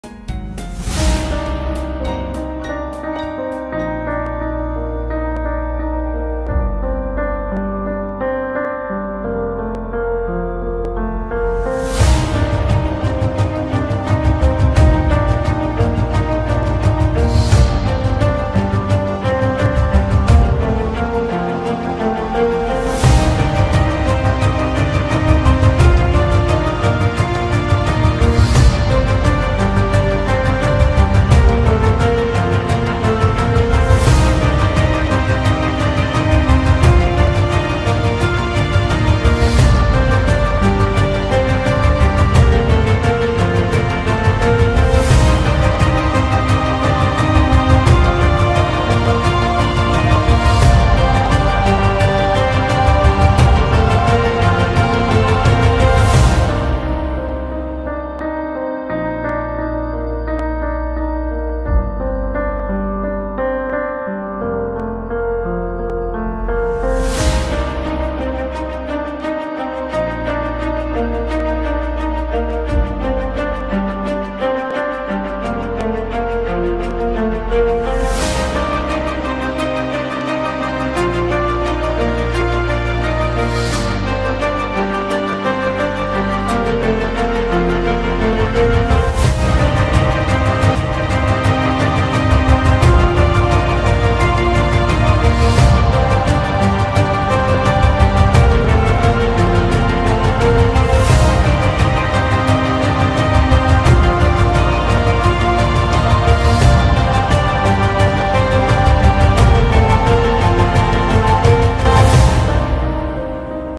Glimpses | Spectrum 2019 | Inspiration School, Kathgodam
Inspiration School's Mega-Event held on 2 Nov 2019